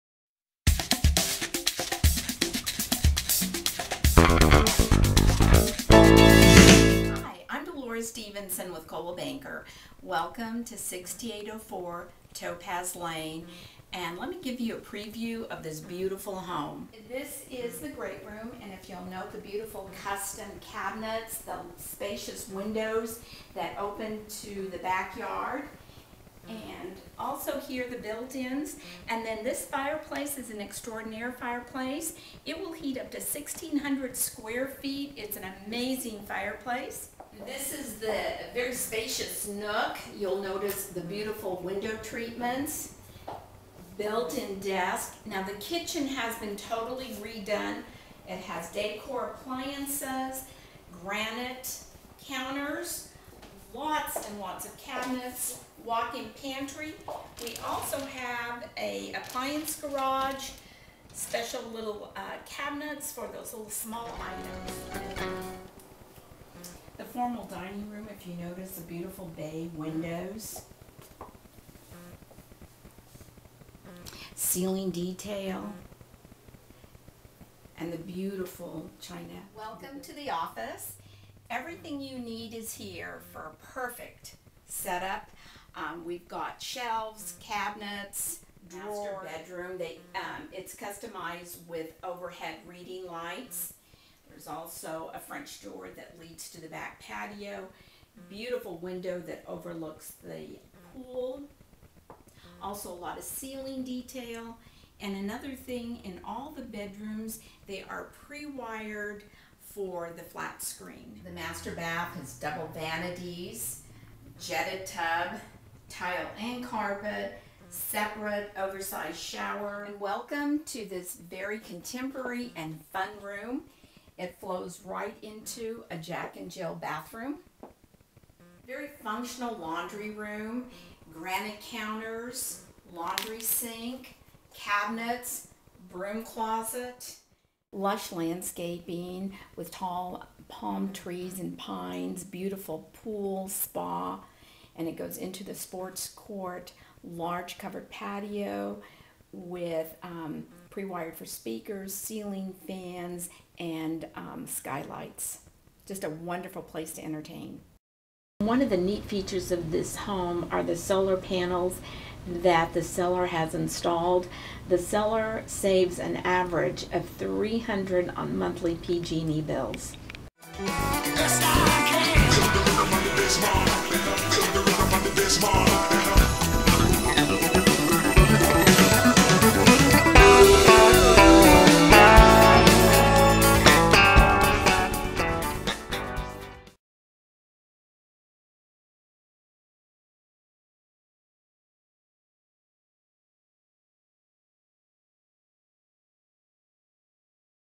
MLS# 21006960 , 6804 Topaz Ln Bakersfield, CA
Emerald Cove Gem. Custom home maintained meticulously. Kitchen has been remodeled with granite counters, cabinets & dacor appliances. Oversized 3 car garage with pass thru door. Lush landscaped yard with pool and an amazing sports court. 3 bedrooms plus an office. 2.5 baths. Plus solar system with savings of over $300.on monthly PG&E bill. Some months the seller will get a credit from PG&E. Great family home loaded with amenities. A great home to show. Walking distance to Park and good schools.
Dolores Stevenson, Realtor
Coldwell Banker Preferred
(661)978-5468

Bakersfield Real Estate Agent